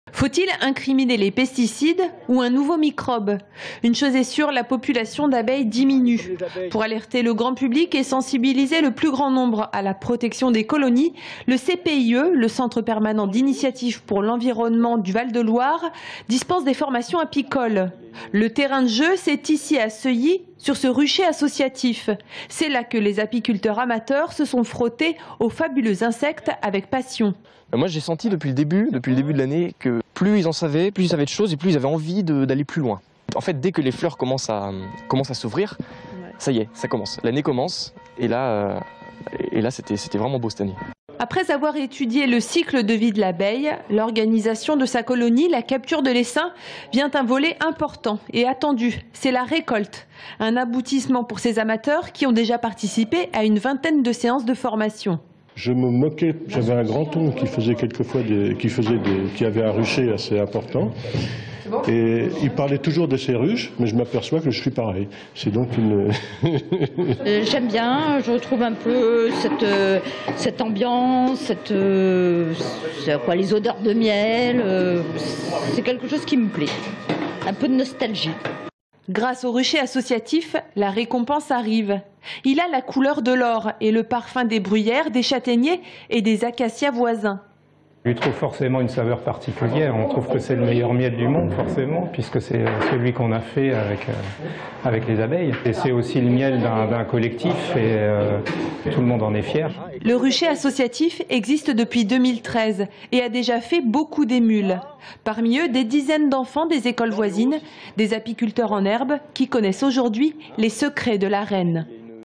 Abeilles: des formations apicoles pour sensibiliser
Sites webs/blogs, copiez cette vidéo avec le bouton "Intégrer" !
 Pour sensibiliser le grand public à la protection des abeilles, le CPIE dispense des formations apicoles.